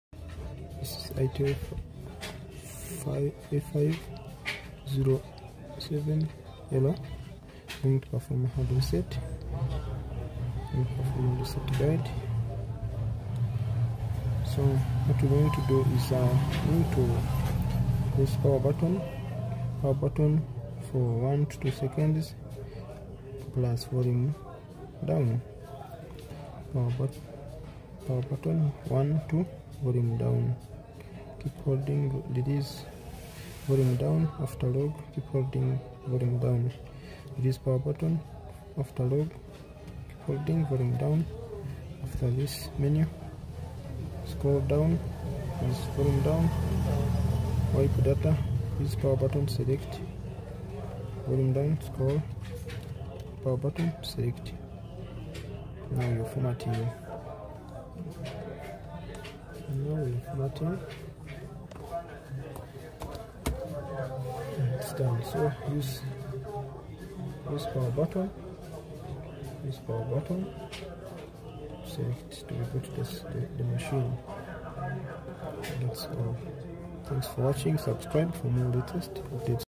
How To Hard Reset A507LM | Itel V52A Factory Reset Guide Latest update 2025
How To Hard Reset A507LM | Itel V52A Factory Reset . here learn how to perform hard  tutorial